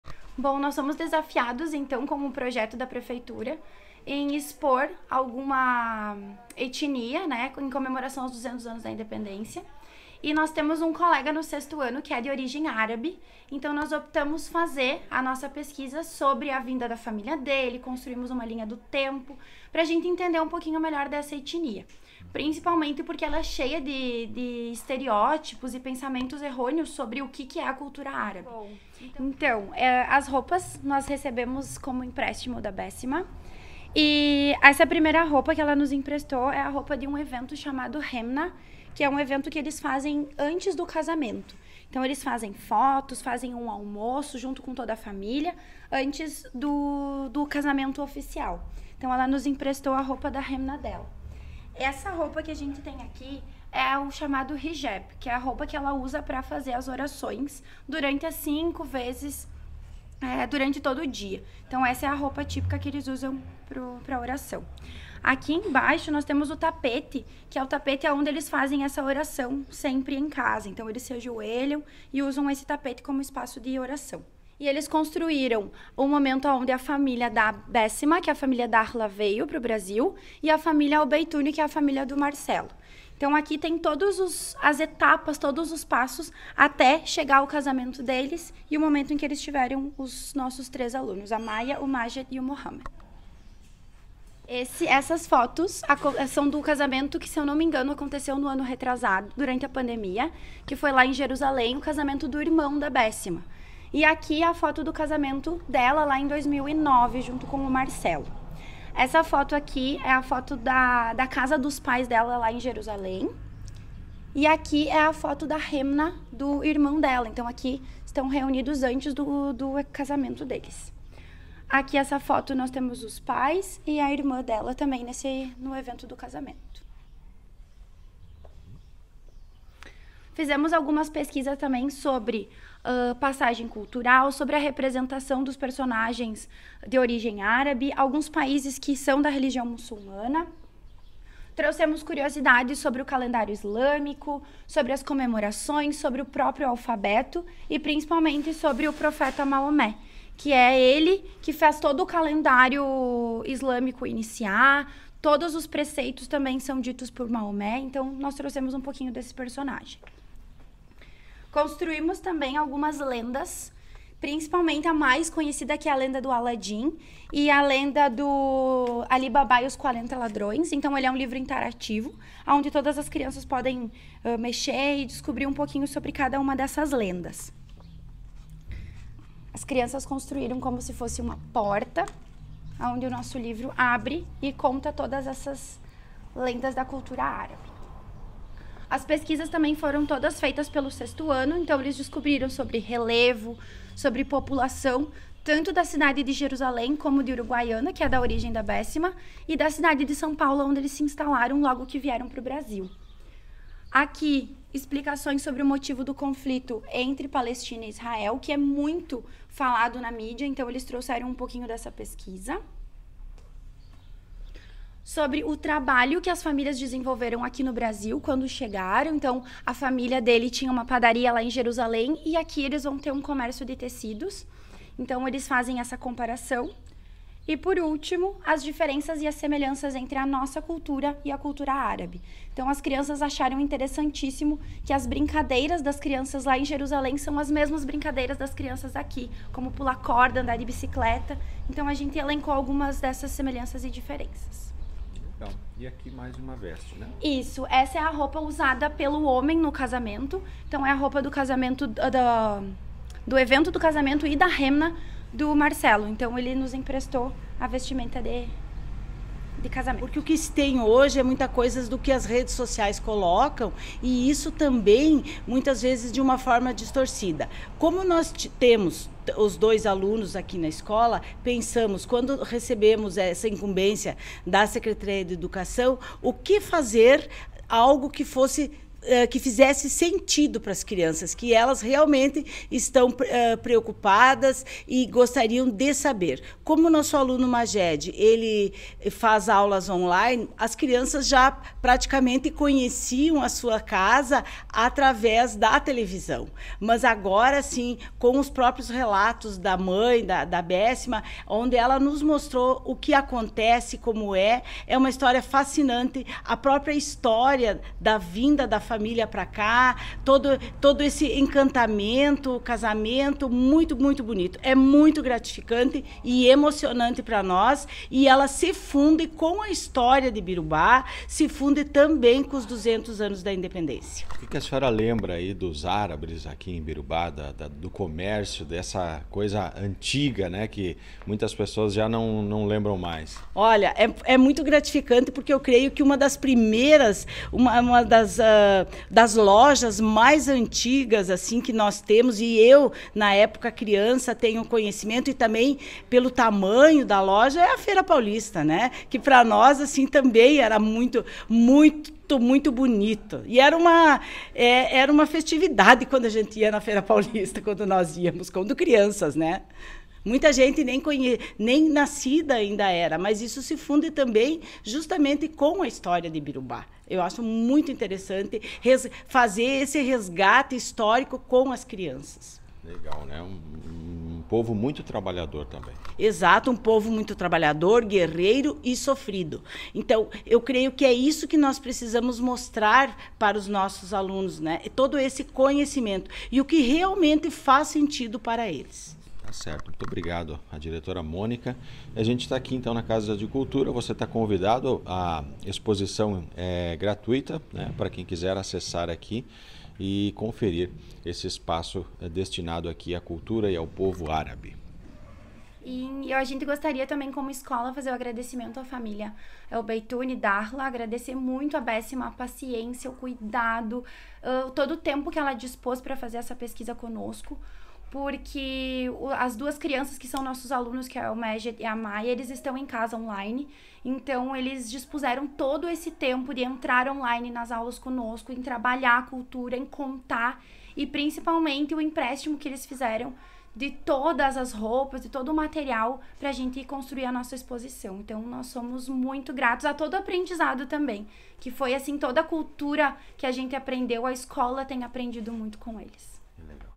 Escola Sinodal apresenta detalhes da etnia árabe em mostra cultural da prefeitura de Ibirubá
Professora de História Mariana Almeida dos Santos e Diretora da Escola Sinodal Mônica Froeder